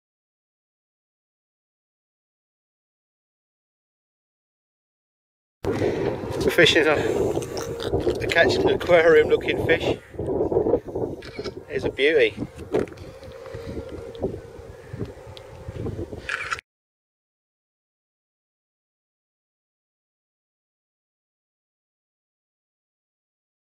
Catching A Pork Fish Cuba Cayo Guillermo
Thanks Guys I had no idea what fish this was.
Thanks for the updates.

This was caught off the causeway between Cayo Coco and Cayo Guillermo Cuba.